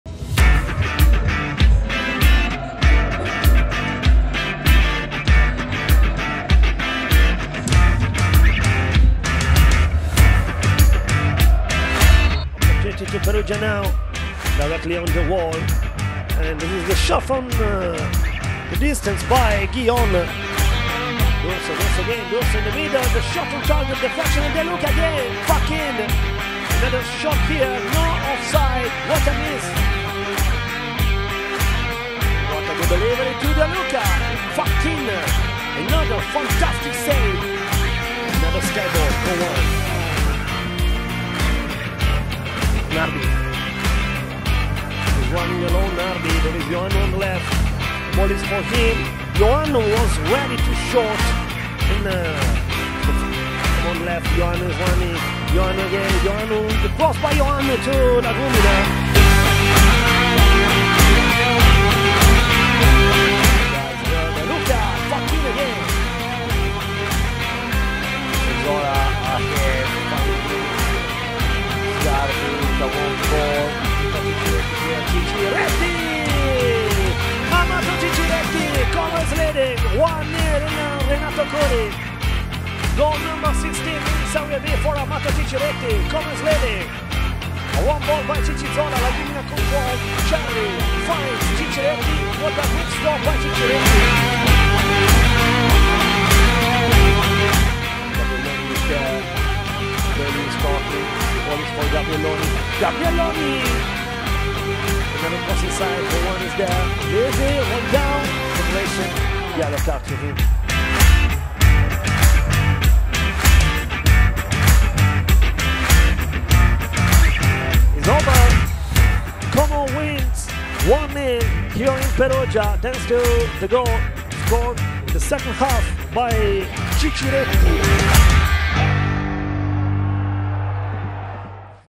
Highlights Pertandingan Perugia 0 - 1 Como [Serie B: 20 Maret 2022]
Akhirnya Como menang! Gol indah dari Amato Ciciretti membawa Como unggul atas Perugia tanpa balasan. Dan lagi-lagi performa apik dari Facchin berhasil menggagalkan segala bentuk serangan Perugia.